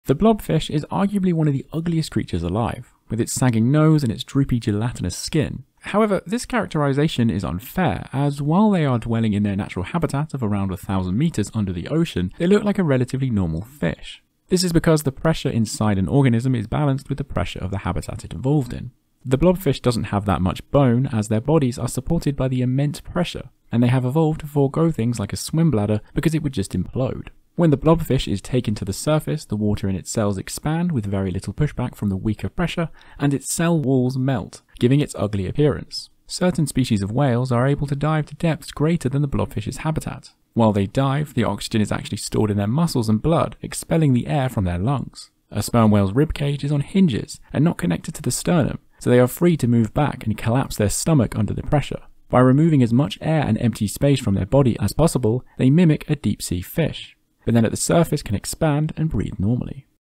Why do Deep Sea Whales not Look like a Blob Fish?
When a blob fish is taken to the surface they deform as their bodies have adapted to thrive at a certain water pressure. Whales are able to transit between different water depths and pressures with no effect, how?